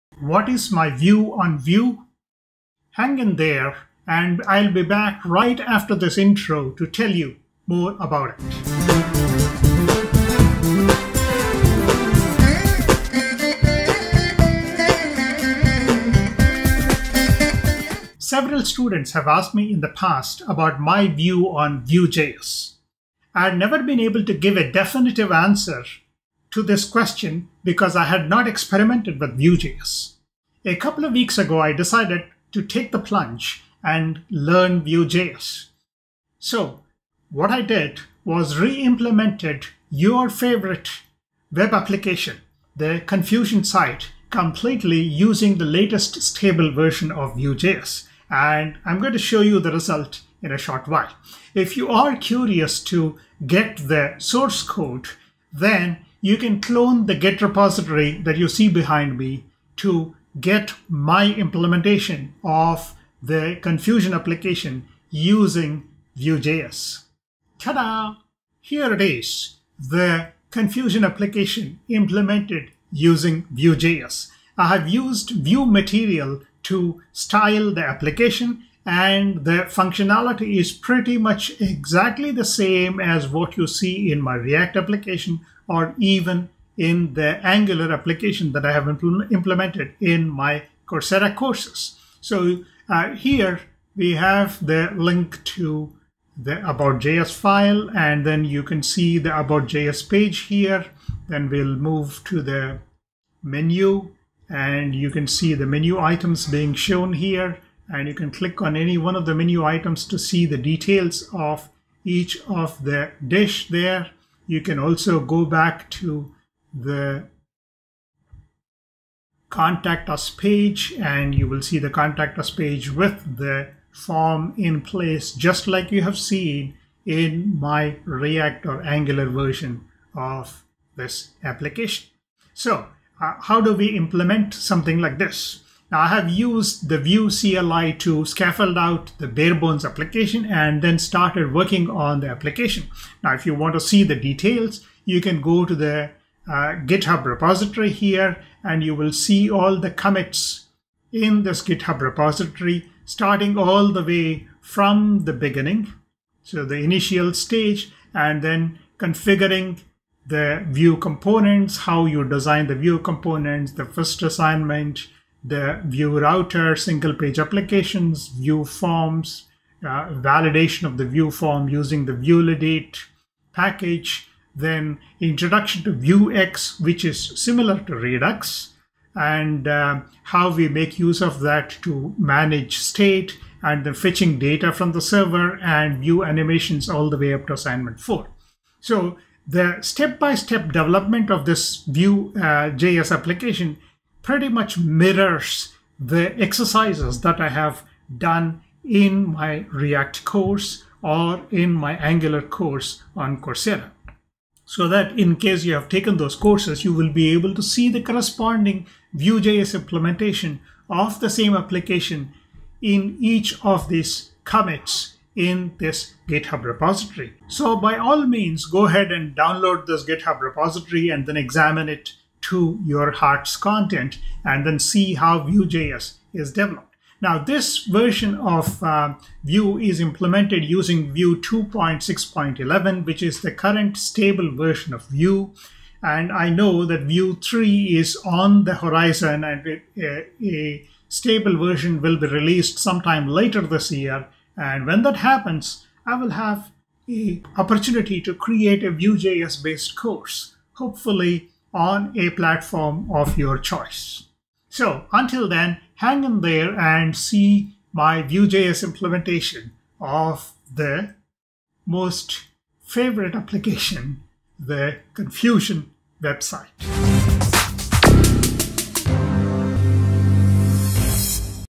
The ConFusion App in VueJS
The ConFusion App Using VueJS: In this video I explain the implementation of the ConFusion App using VueJS.